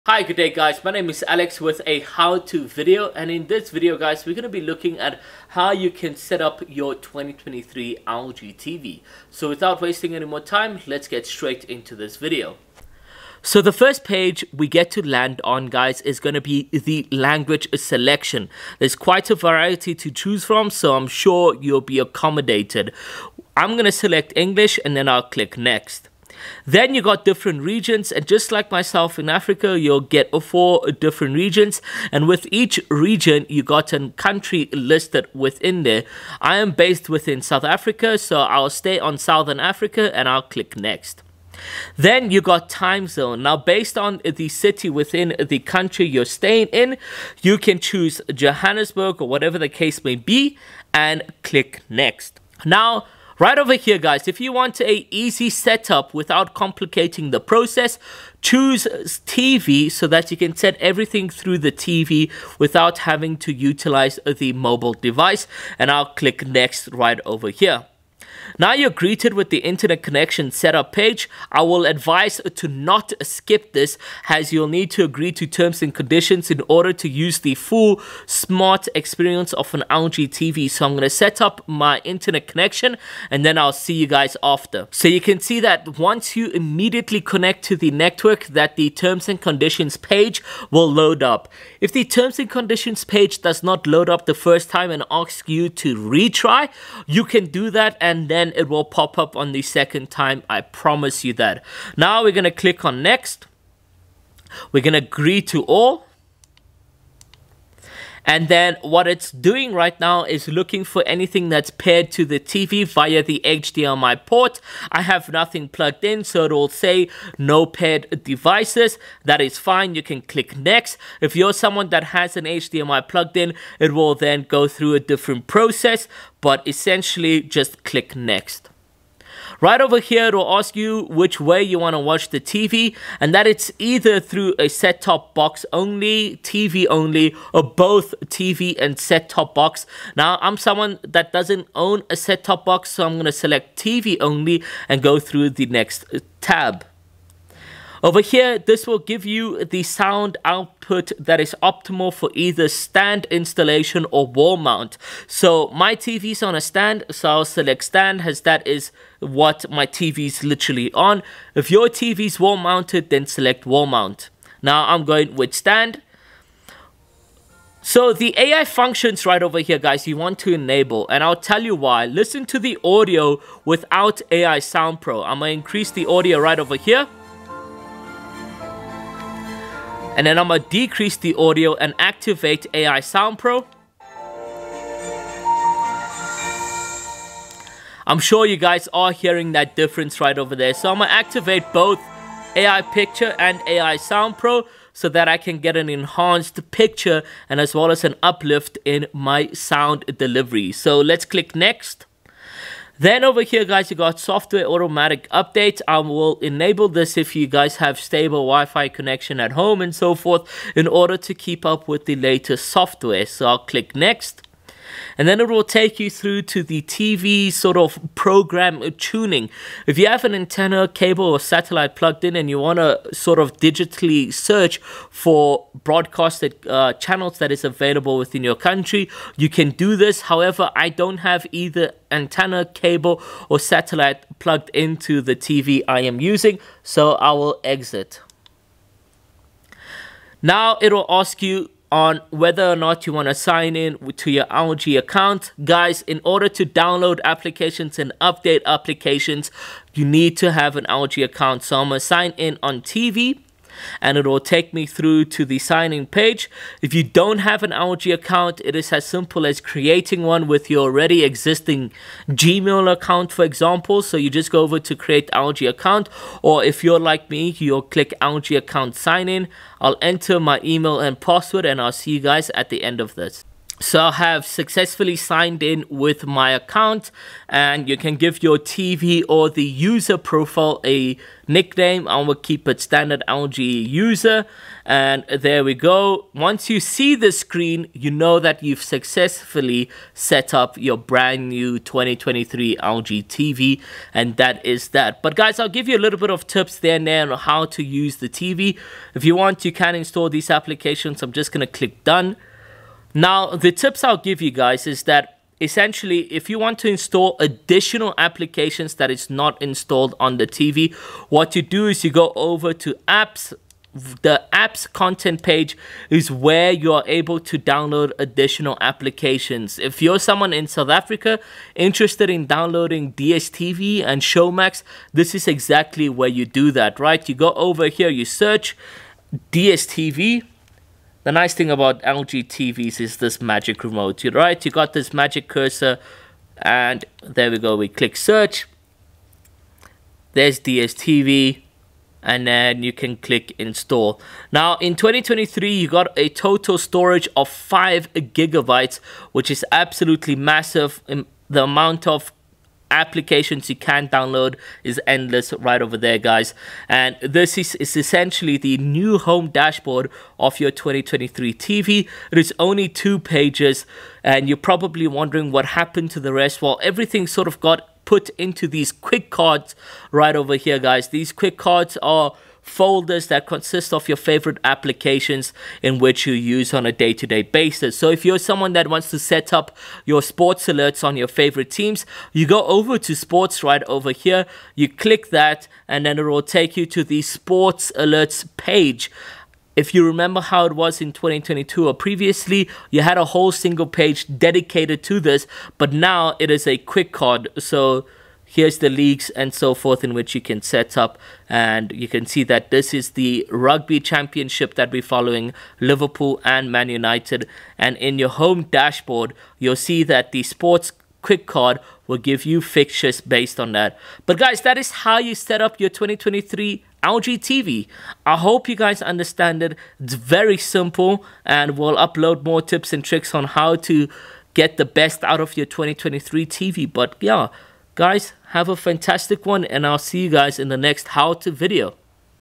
How to set up your 2023 LG TV | Install Additional LG Apps | LG OLED
In this video we go through the installation process as well as how to install apps found on LG App content store.

Please note this process is best suited for Southern African Countries but may still be followed in other parts of the world.